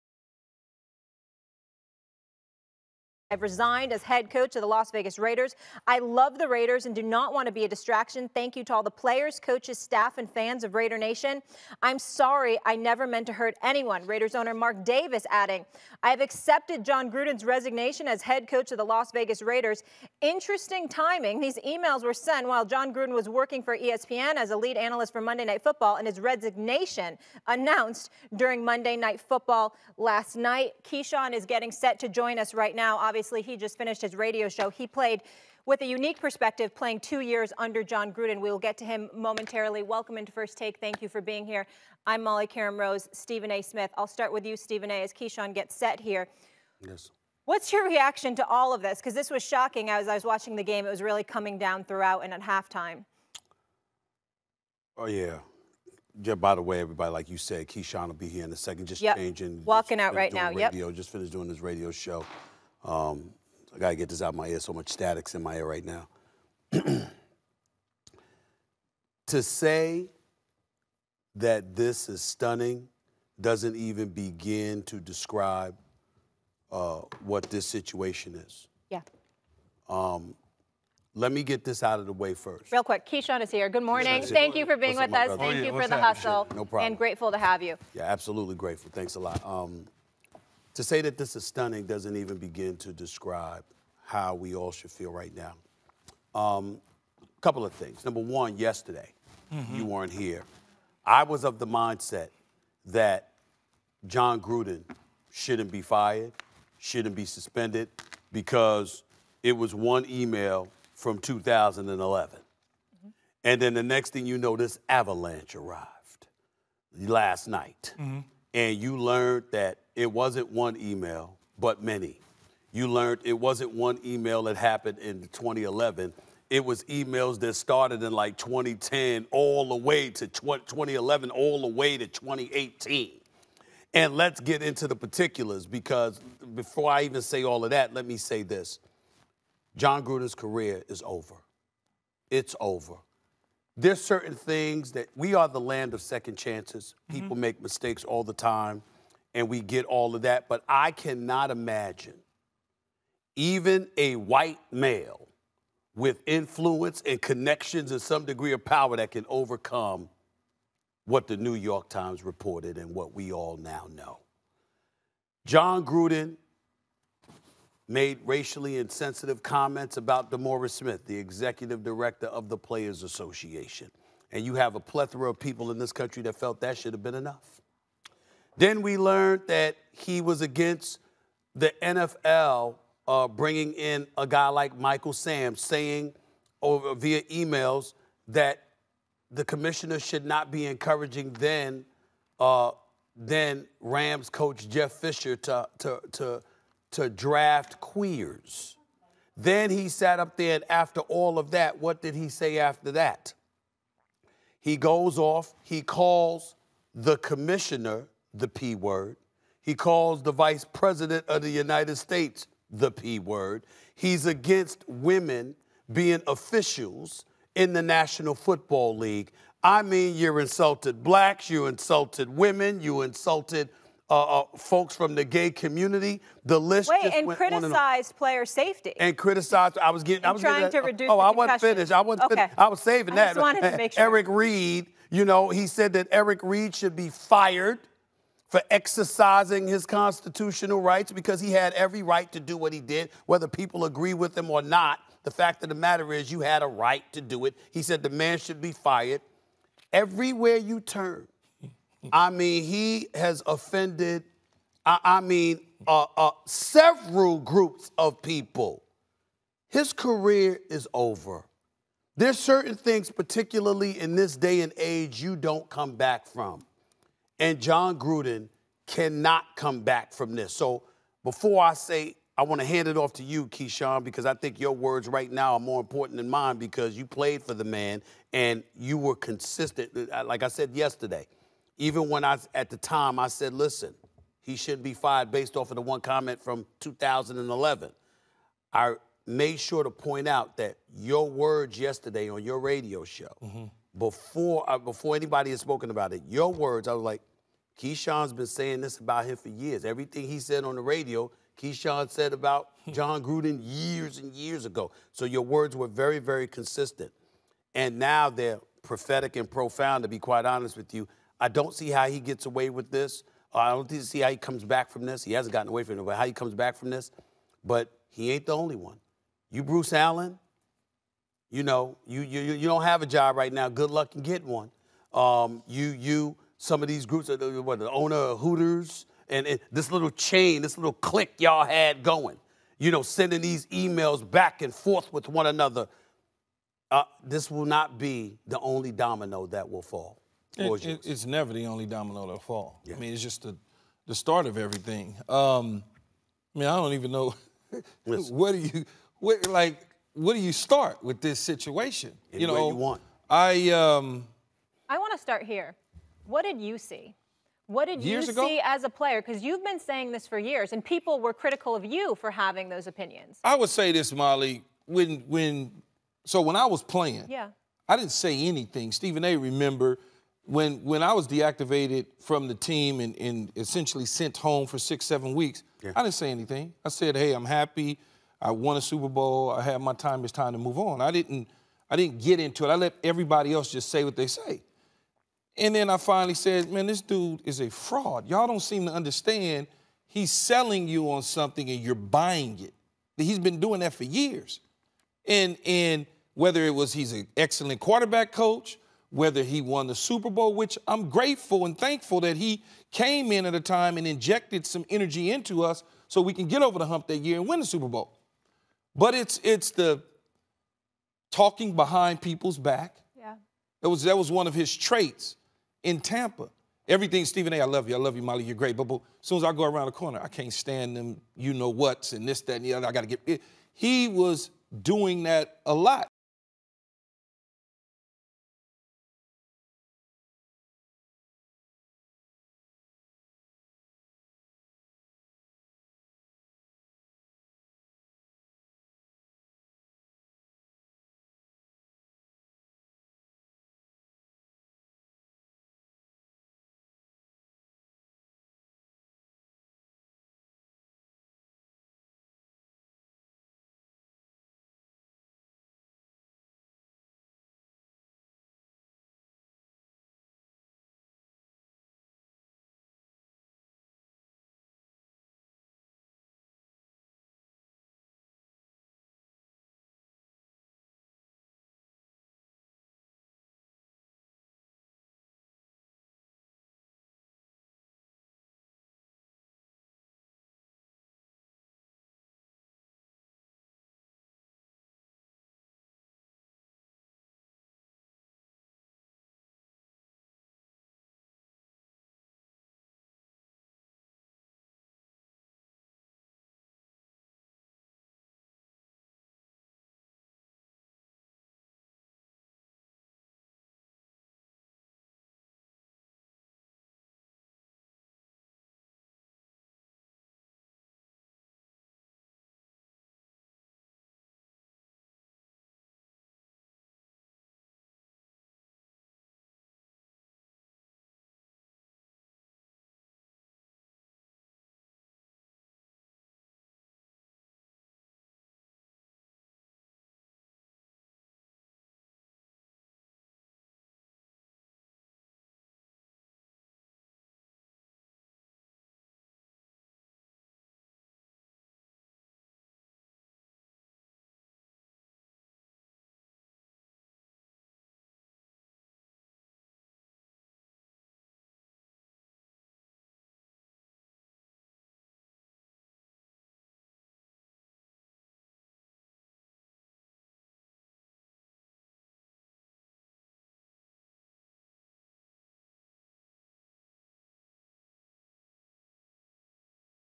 Jon Gruden Resigns as Raiders Head Coach. Stephen A's Reaction!
After his emails regarding racism and homophobia were made public, Jon Gruden resigns. Stephen A. Smith reacts.